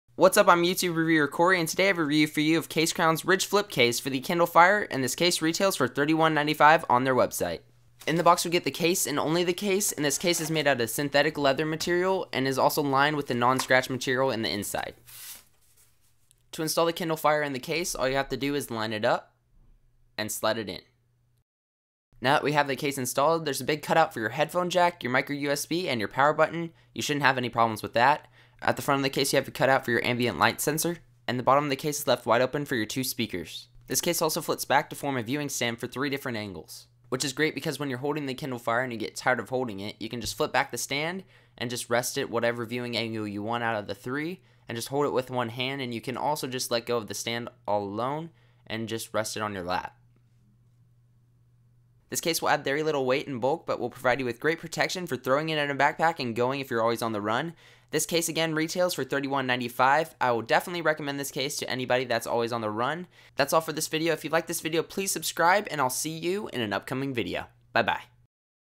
Review CaseCrown Ridge Flip
Looking for at the Ridge Flip case? Or looking for a Kindle Fire case make sure you watch my review before you make your decision!

Camera: Canon 60D
Lens: 24-70mm L
Editing: Final Cut Pro X